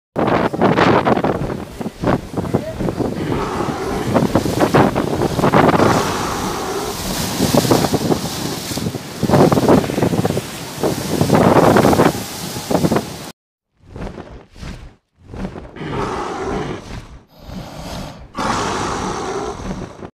PTEROSSAURO NA PRAIA
Neste vídeo, fiz mais um teste com recurso chroma key, peguei alguns vídeos  prontos de dinossauros, com o fundo verde, e juntei com um vídeo da minha galeria.